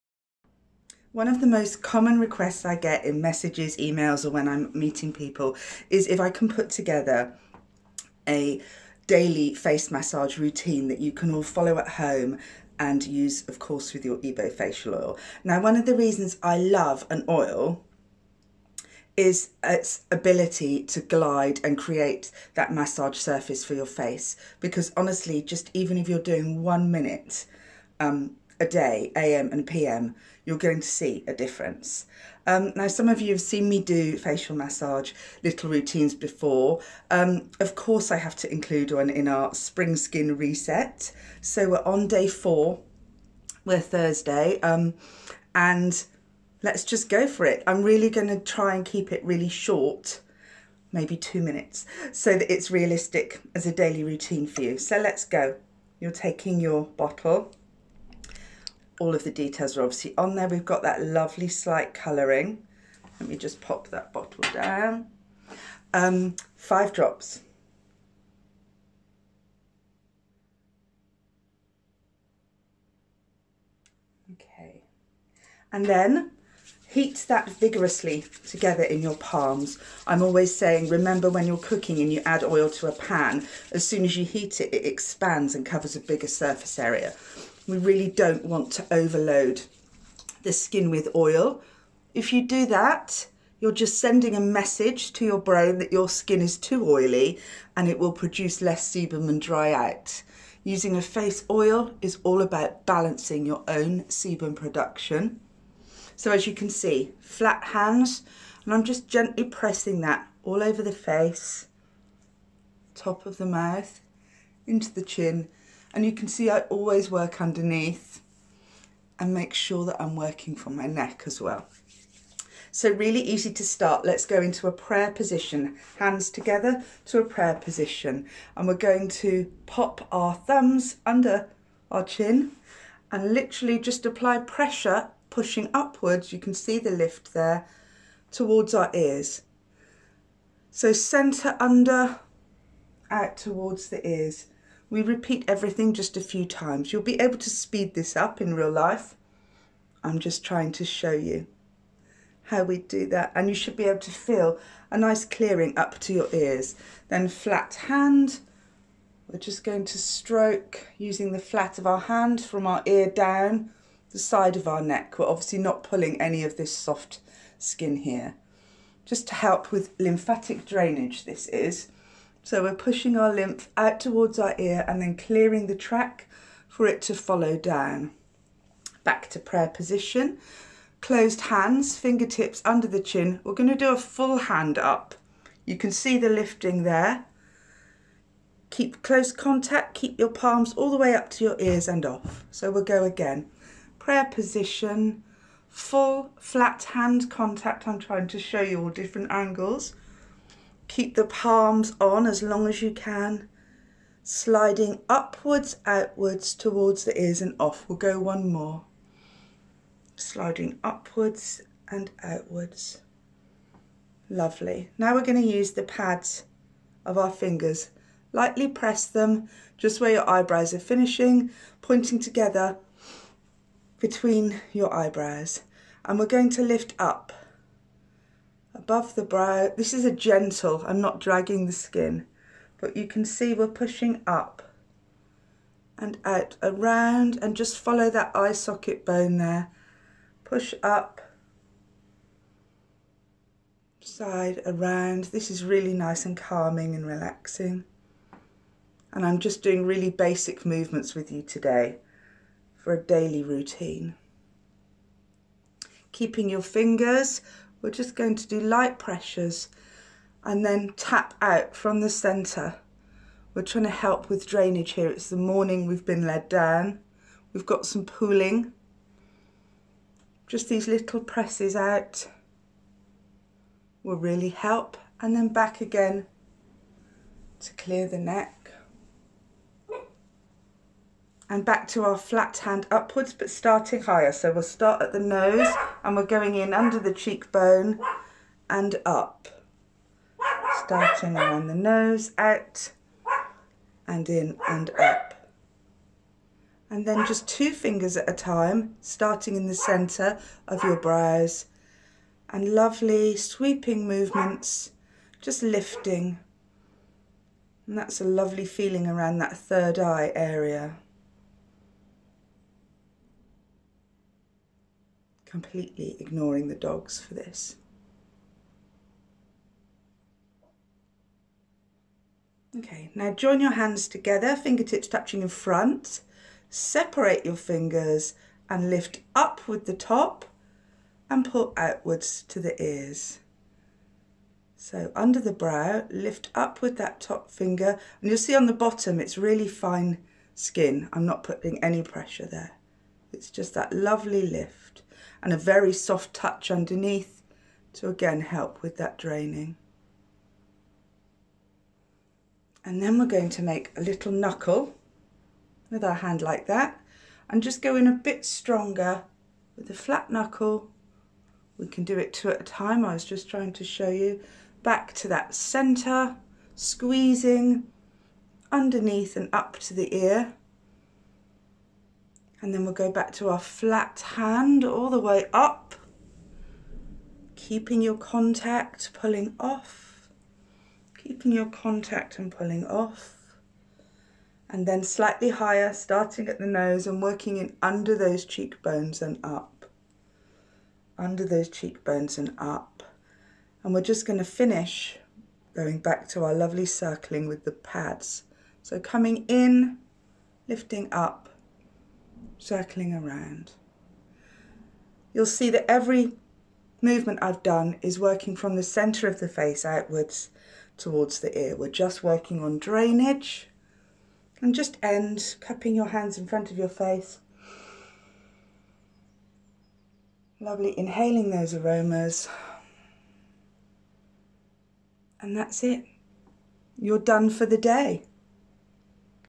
Face Massage how to massage face oil daily for lifted and plimpres skin
A really quick and simple to follow face massage demo. How to massage your face daily when applying face oil.